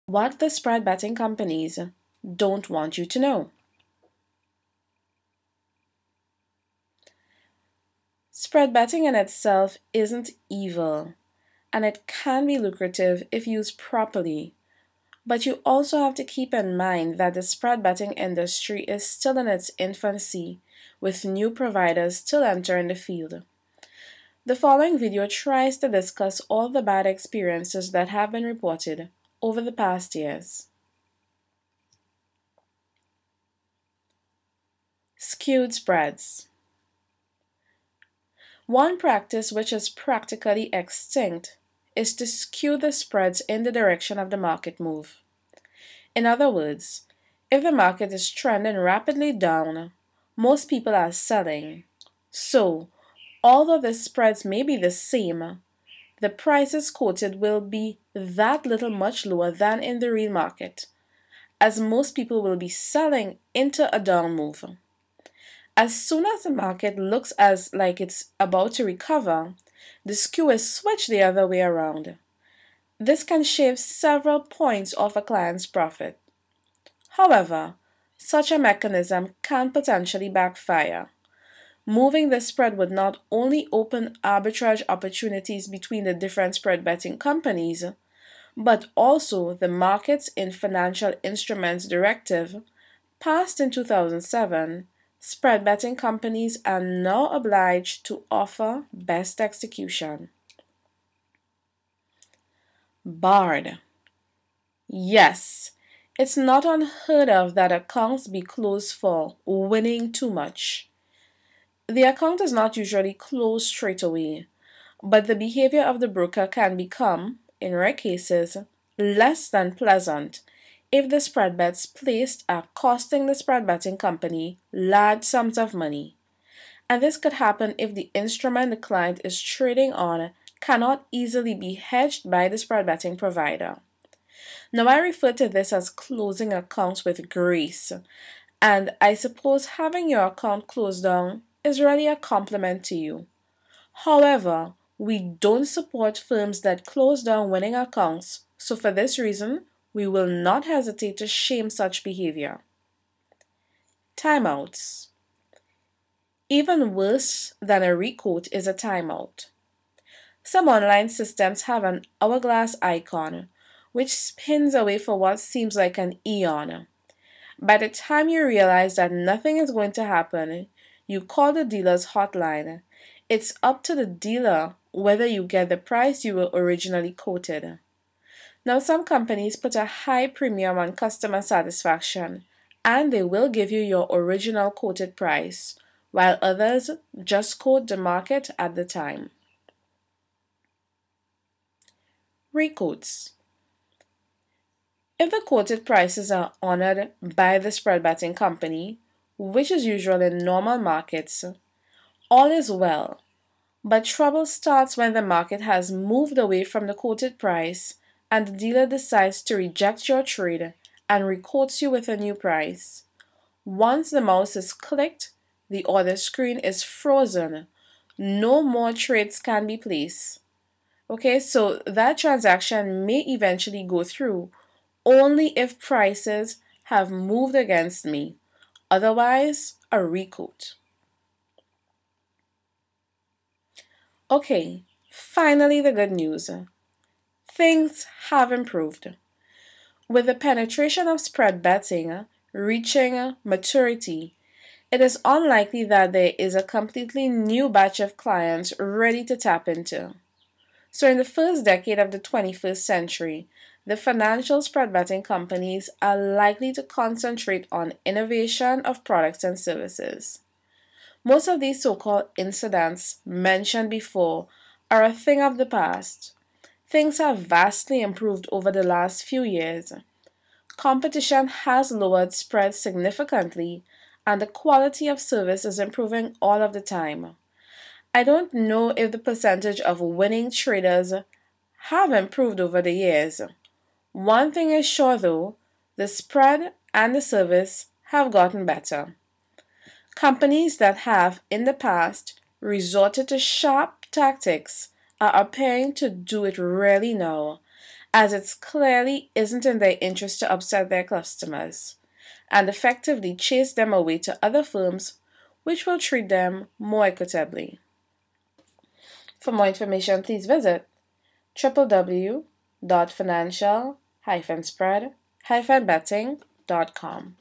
What the Spread Betting Companies do not want you to Know!
Spread Betting in itself isn't evil and can be lucrative if used properly but you also have to be keep in mind that the spread betting industry is still in its infancy with new providers still entering the field. Cases of bid-offer spread spikes and skewed spreads are not unheard of and people barred because of winning too much.